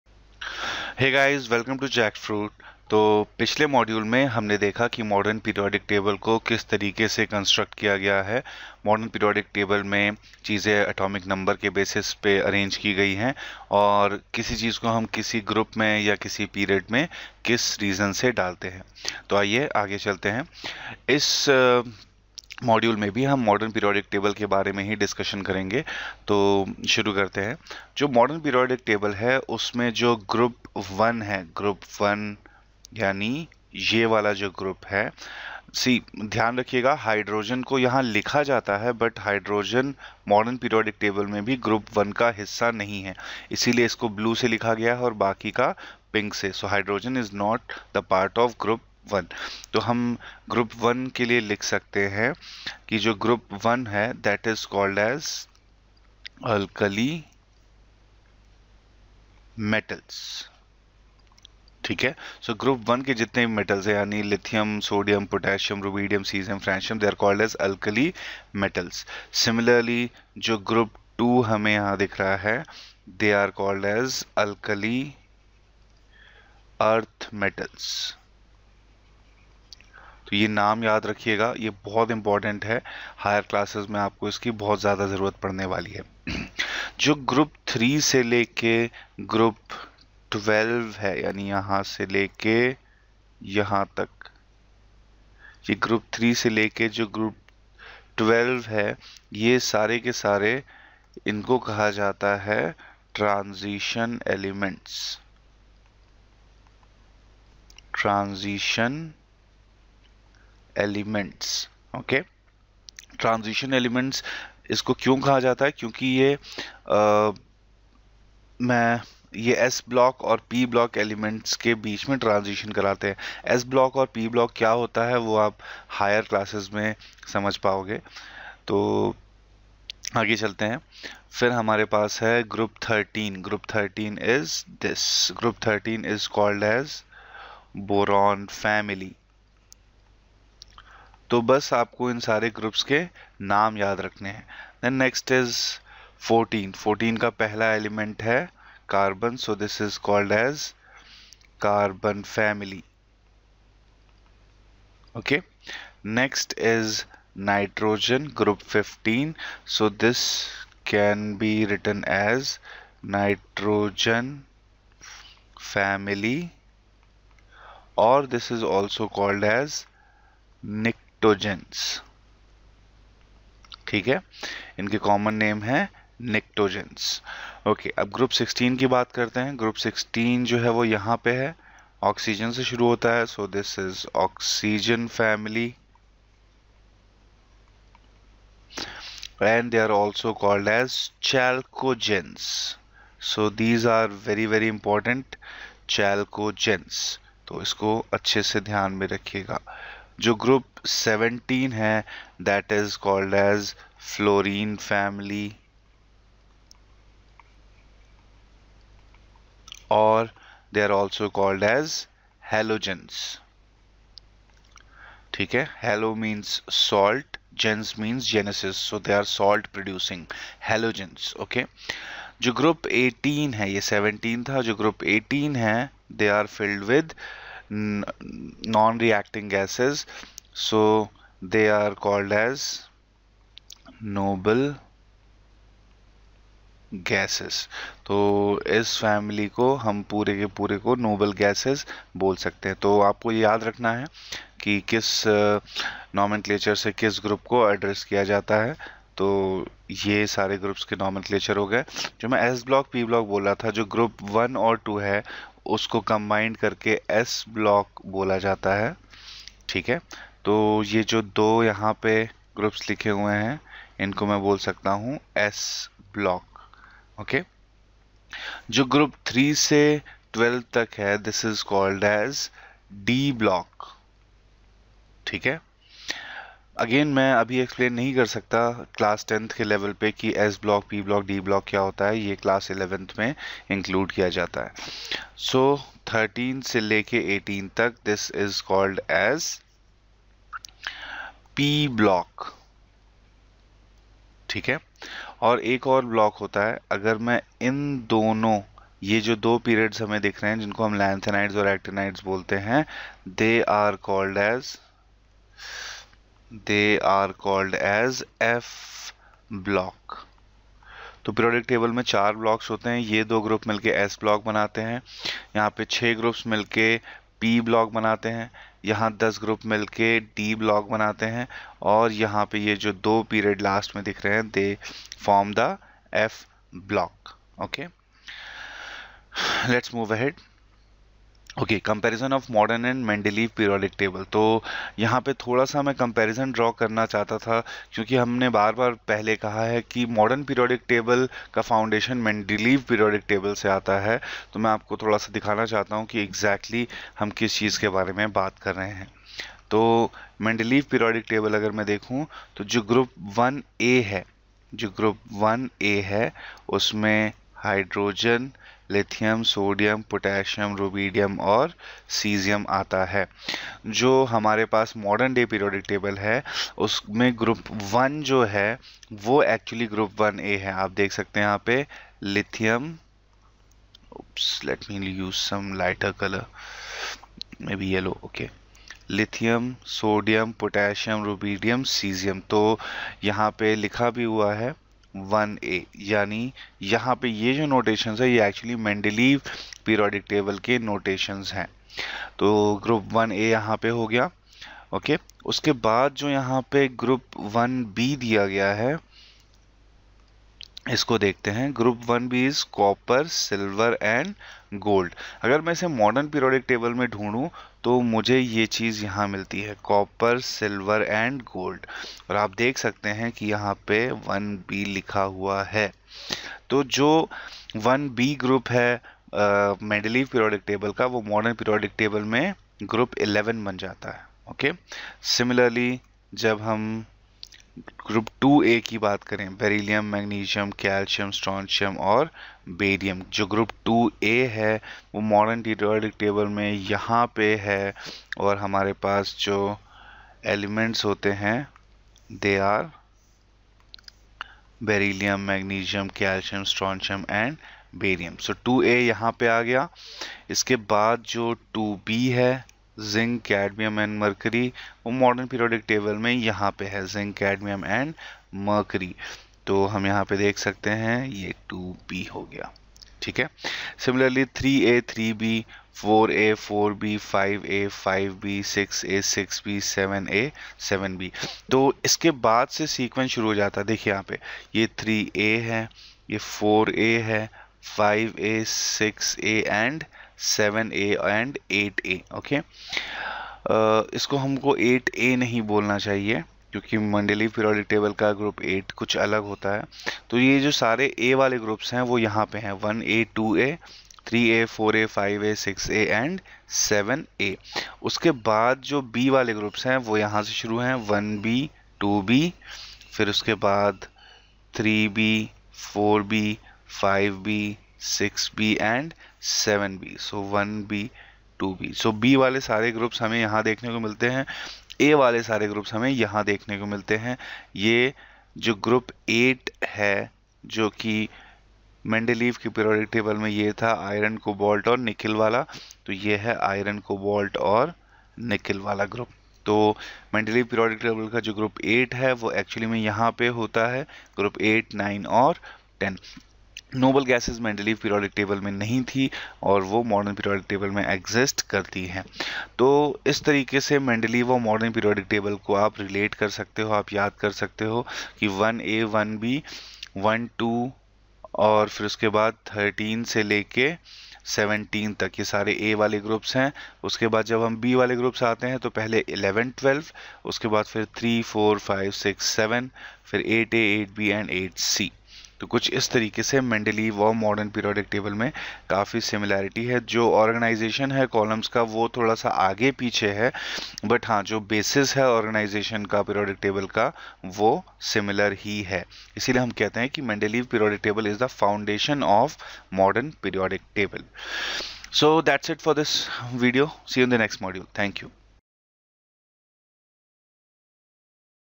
Modern Periodic Table - Part 2 | Periodic Classification of Elements | Hindi
In this video, we have discussed the modern periodic table part 2. Here we provide class 10th learning materials and NCERT Solutions for free.